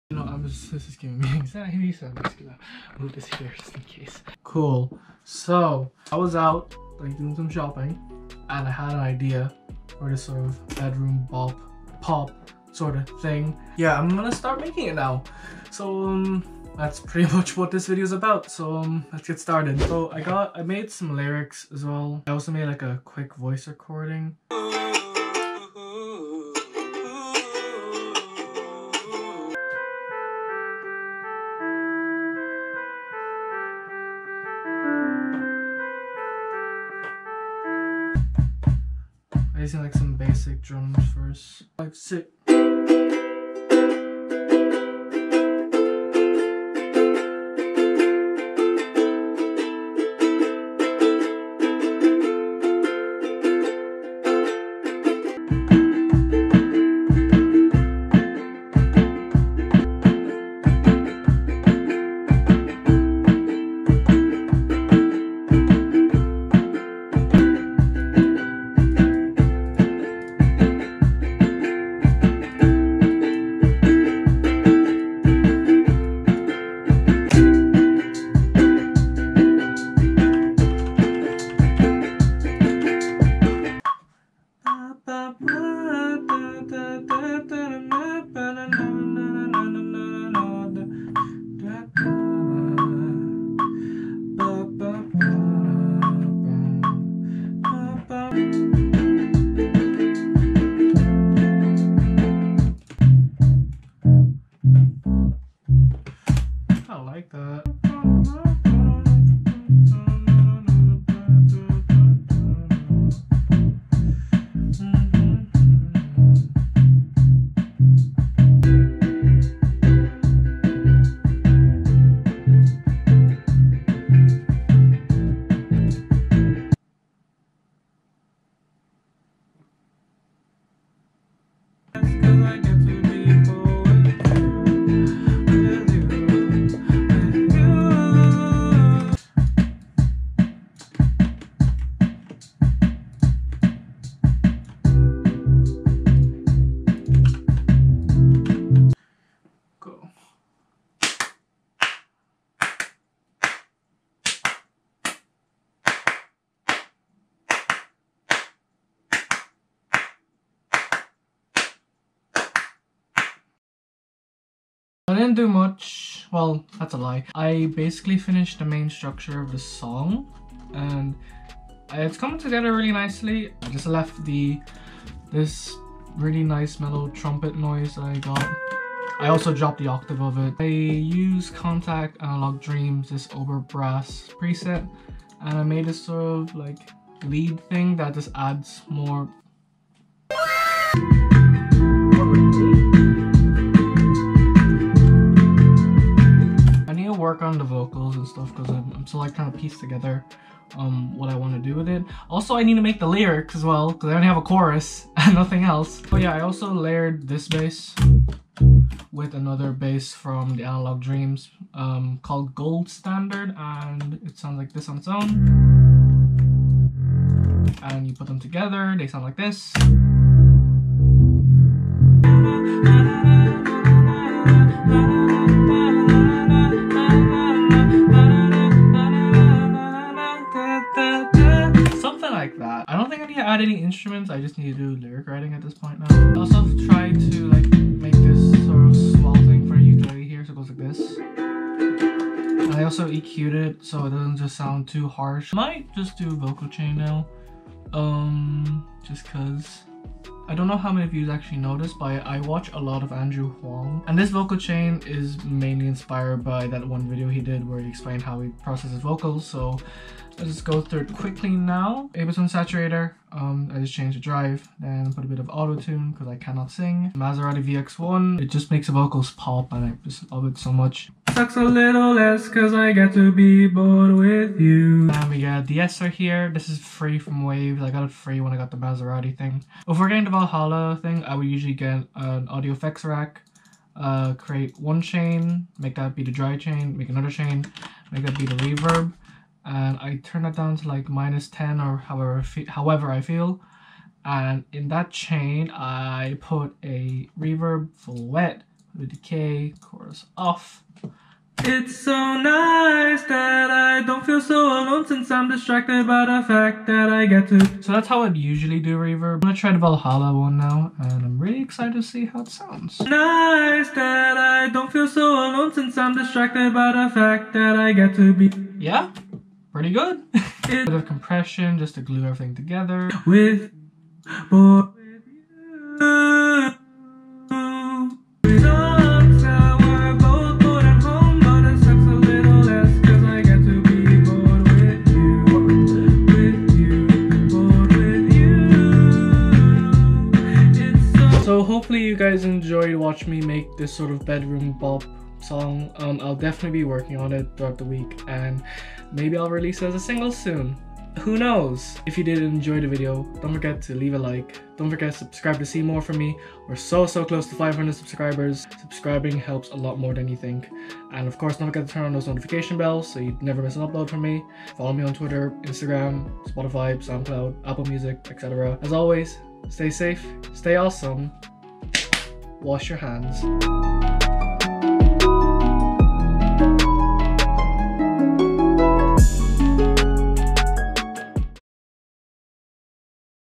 Making a bedroom pop/indie song FROM SCRATCH
Today's video is a bit of a mess. Firstly, I lost some footage so thats very big sad. Secondly, it's a video about how i created this sorta bedroom pop/indie song after coming up with an idea the day before. So, I hope you do enjoy it and keep a look out for when I (eventually) release this as a new single!

Setup information:
My microphone is the Rode NT1A and I record and mix everything with Ableton Live 10